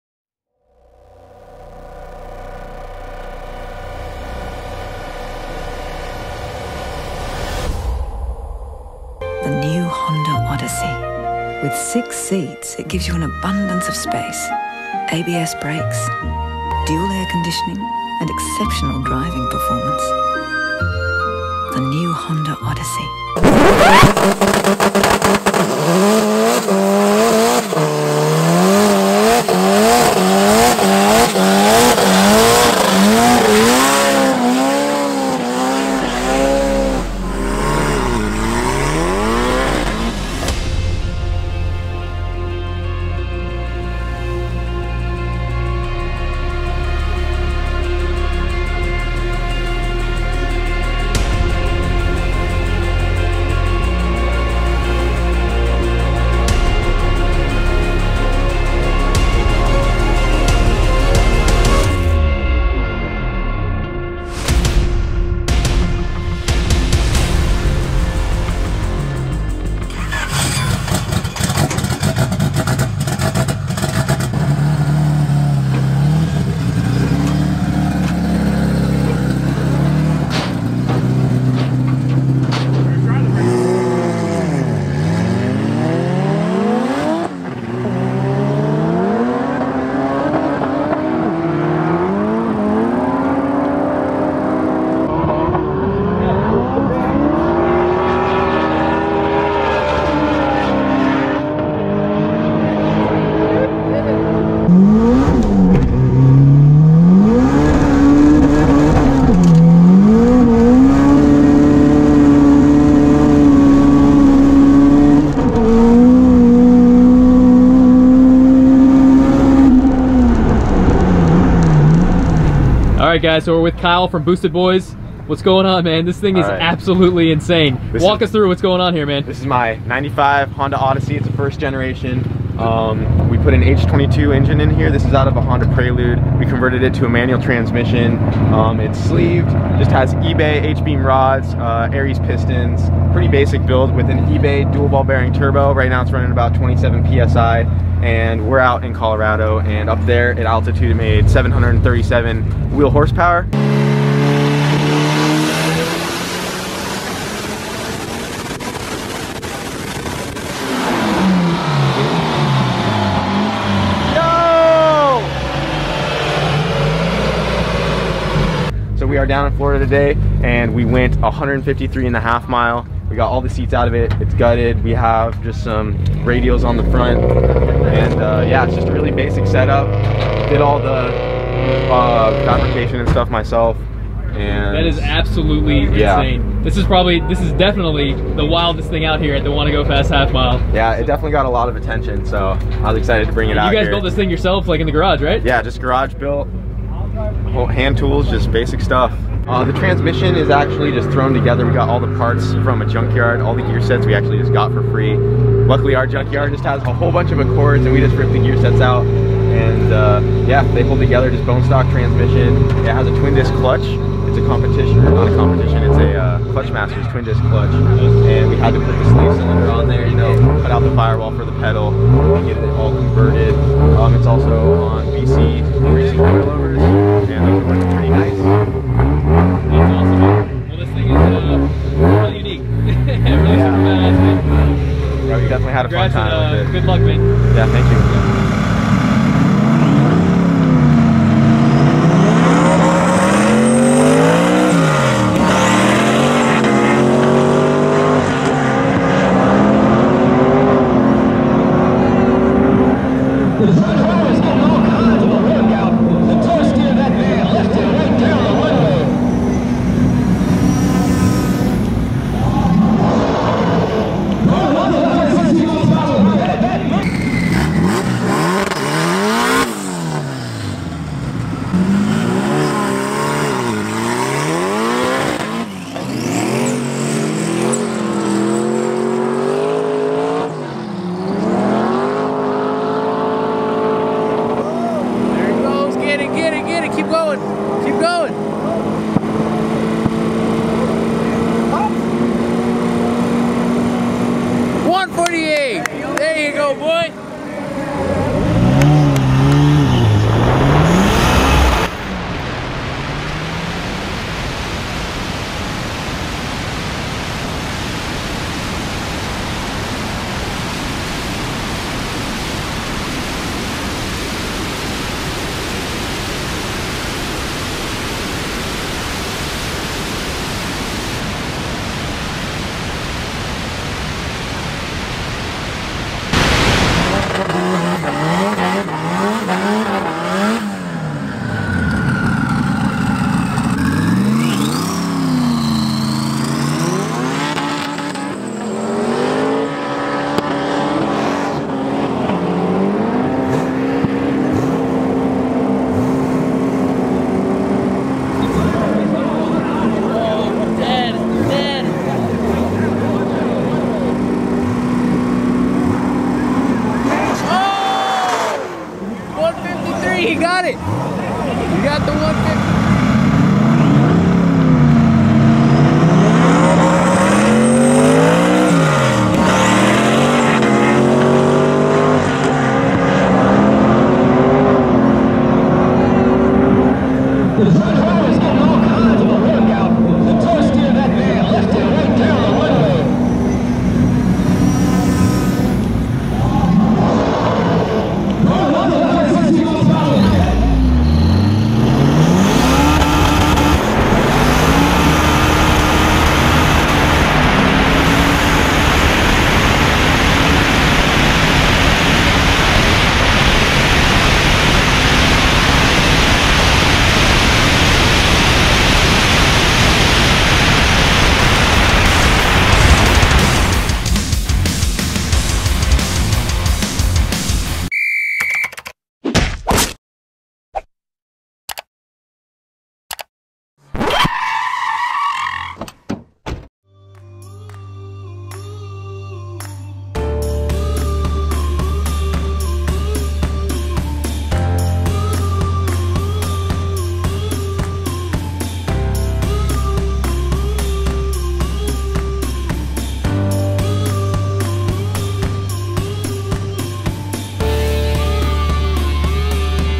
700HP Honda Odyssey Minivan?! BoostedBoiz!
This might just be one of the BADDEST minivans ever! If you haven't already, go check out @BoostedBoiz YouTube channel to follow the minivan project.